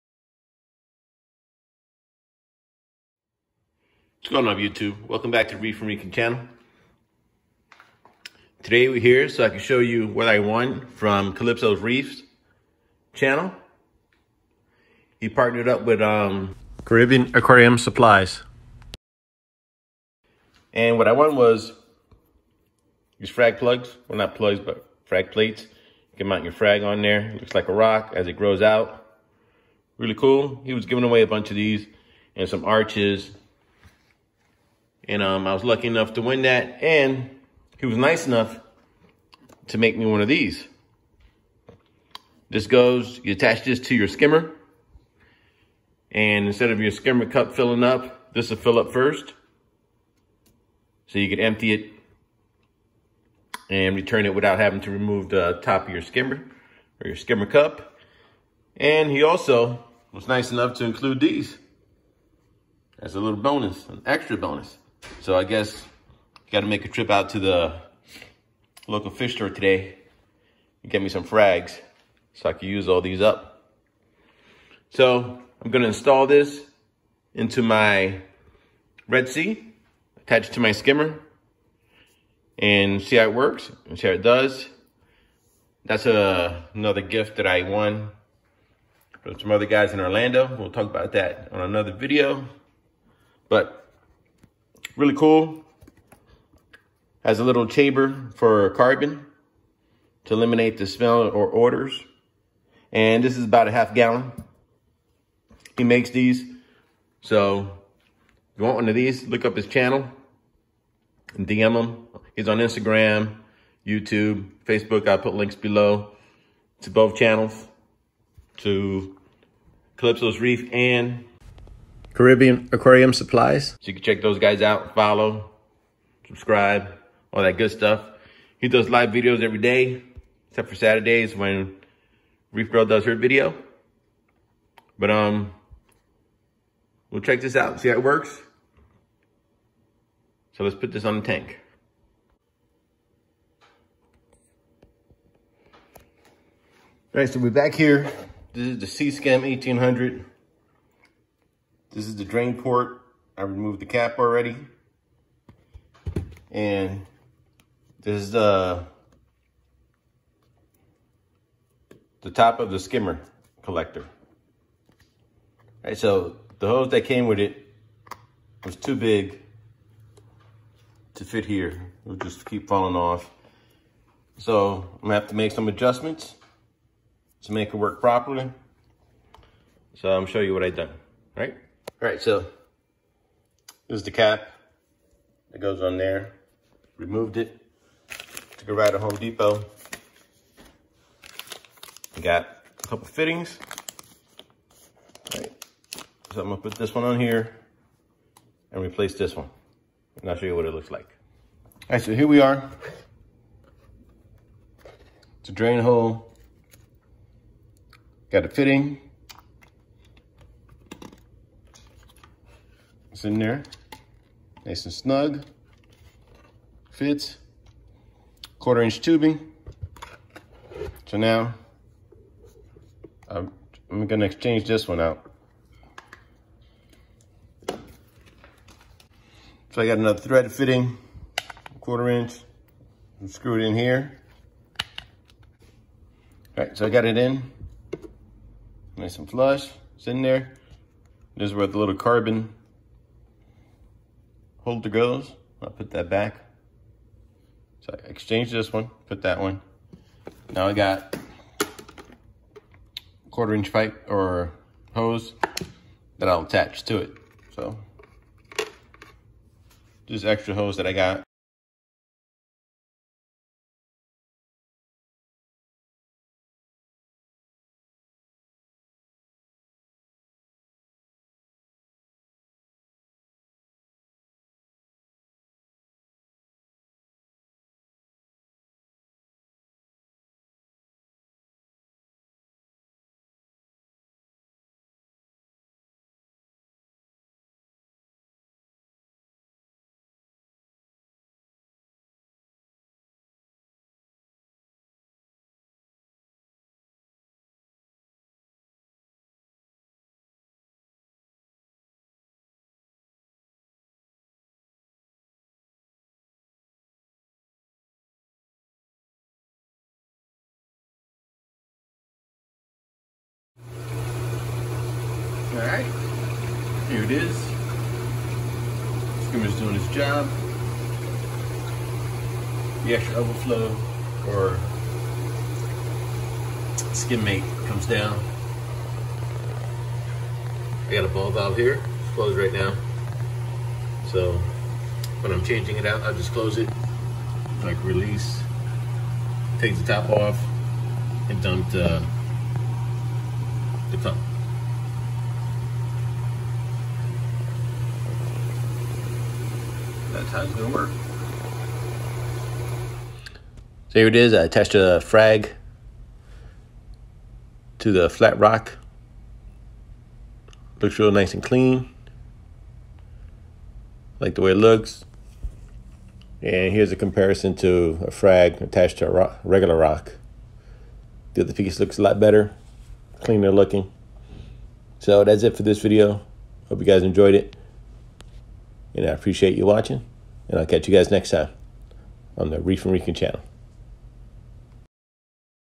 Look at what I won from Calipso’s reef’s
In this video I will show you what I won from calipso’s reef’s  giveaway . I will be installing it on my Red Sea max s 650 . It works with the sea skim 1800 skimmer as an additional skim mate container.